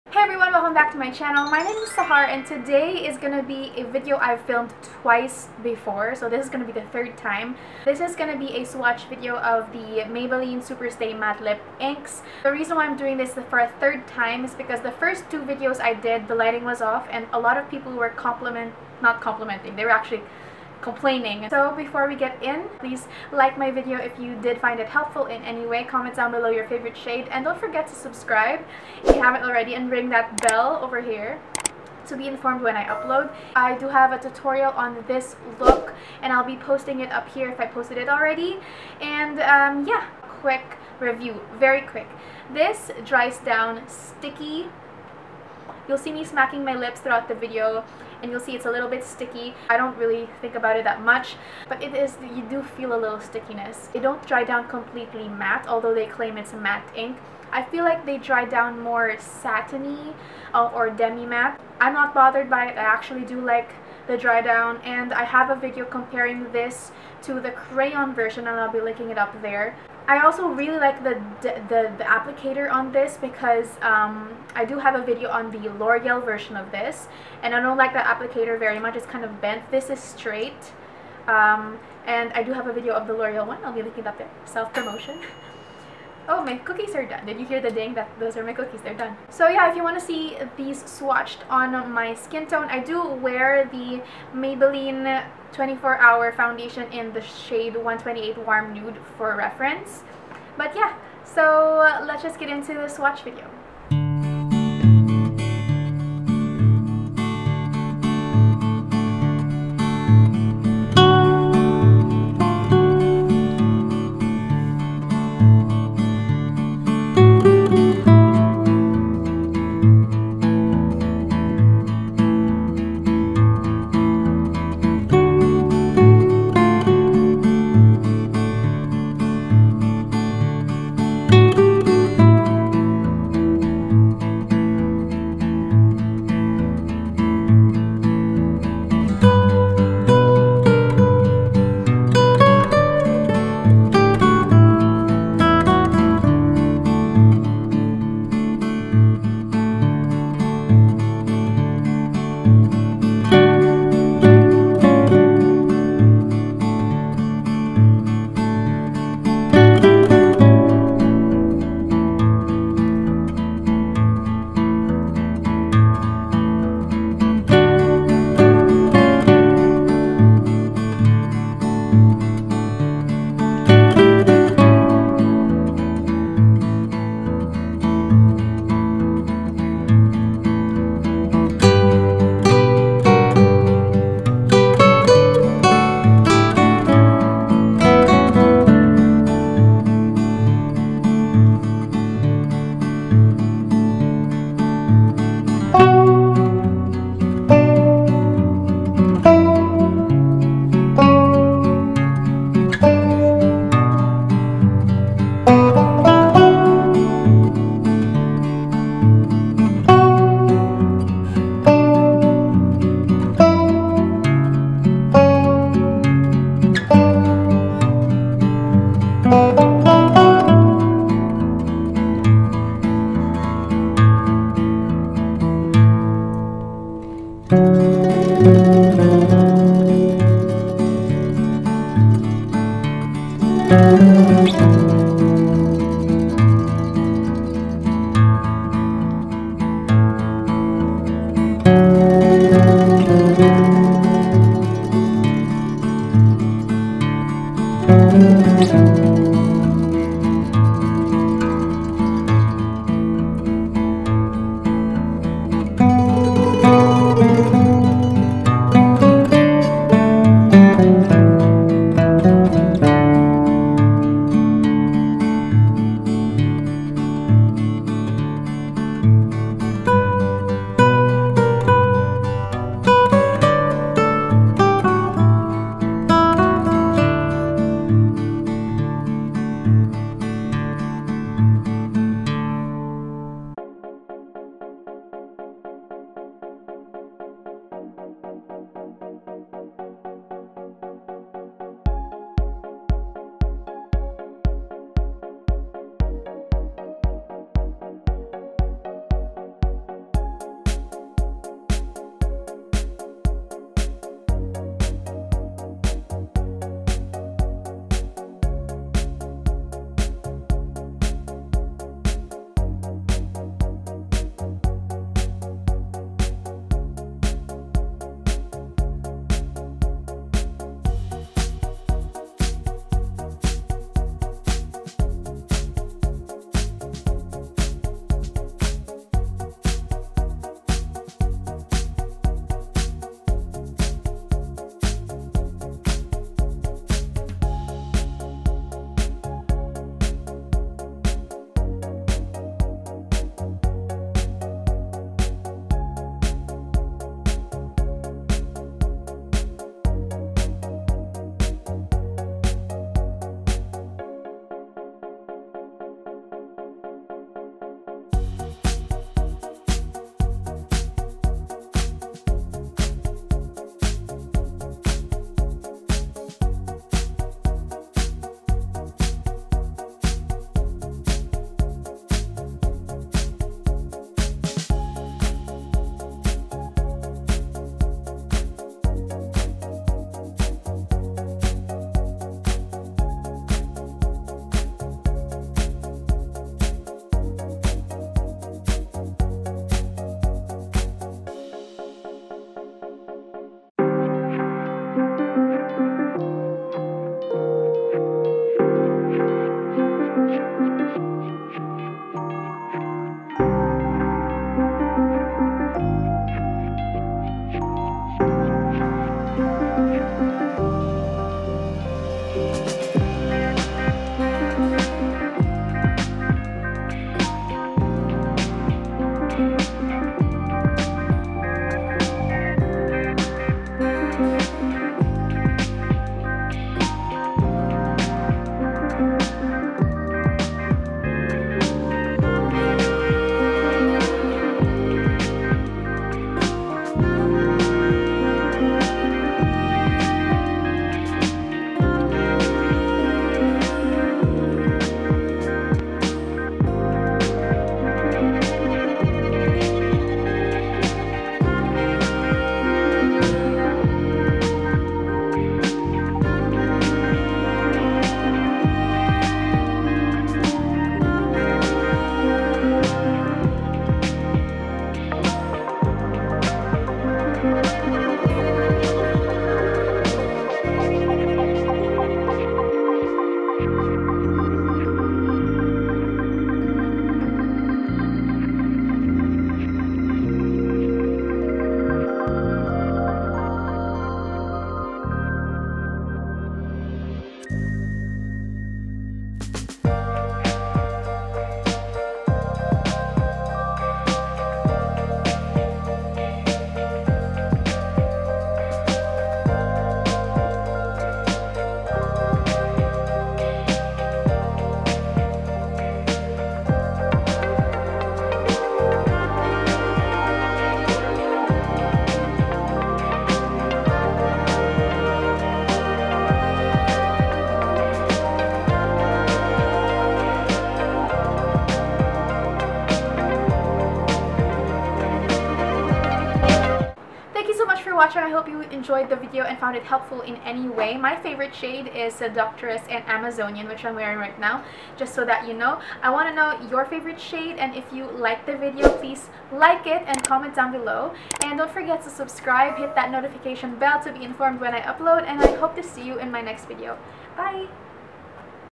MAYBELLINE SUPERSTAY MATTE INK | SWATCHES & REVIEW |BETTER LIGHTING
HI!! THIS IS THE NEW AND IMPROVED VIDEO!

swatches start at 1:44

Hi! In this video, I arm and lip swatch 18 different shades of the Maybelline Superstay Liquid Matte Ink Lipsticks!

Any constructive criticism to make my channel BETTER is very much welcome! Hateful comments will be deleted. Let's spread the love ;)

Credit:
Bought at Maybelline Shoppee Account, SM Dept Stores and Watsons

Maybelline Superstay Matte Lip Ink Lipsticks - $ 9.49